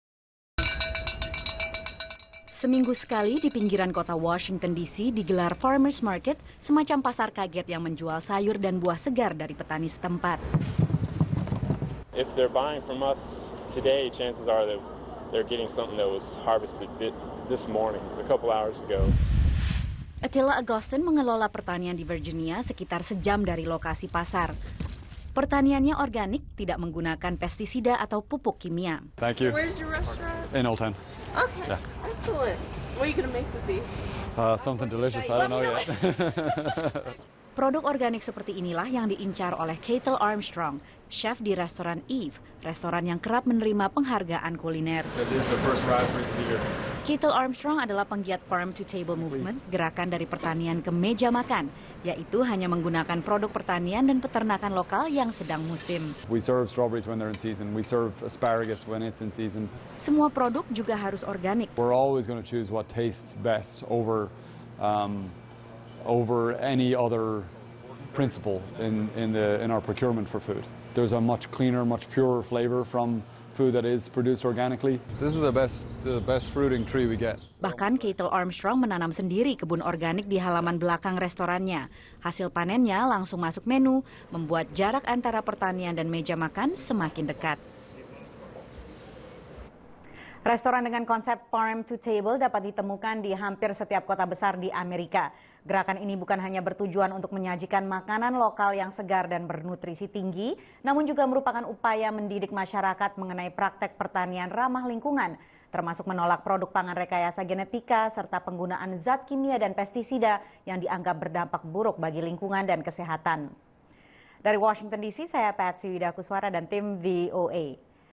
Gerakan 'Farm to Table' di AS - Laporan VOA 15 Agustus 2013
Beberapa tahun terakhir di Amerika Serikat makin populer konsep 'Farm to Table', di mana chef restoran dan penggemar kuliner lainnya menggunakan hanya makanan yang diproduksi secara lokal dan ramah lingkungan. Gerakan ini bukan hanya bertujuan menyajikan makanan segar dan berkualitas namun juga untuk mendidik masyarakat akan praktek pertanian dan peternakan ramah lingkungan dan berkesinambungan. Selengkapnya ikuti laporan reporter VOA Patsy Widakuswara dari Washington, DC.